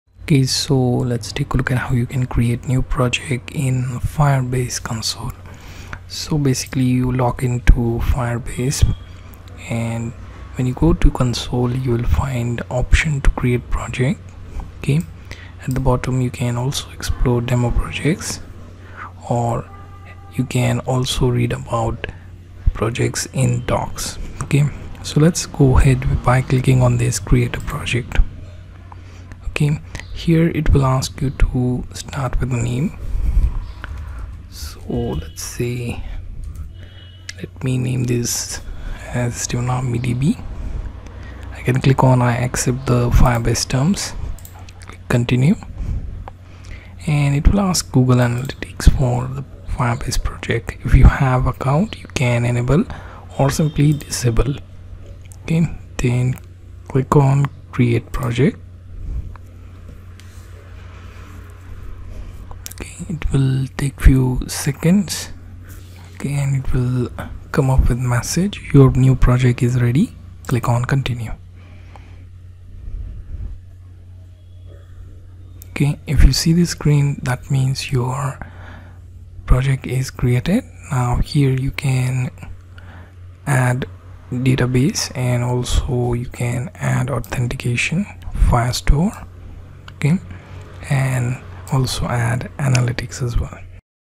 Firebase - How to Create New Project in FireBase
Learn How to Create New Project in FireBase. Learn How do I create a project in firebase. Creating Your First Firebase Project in the Firebase Console.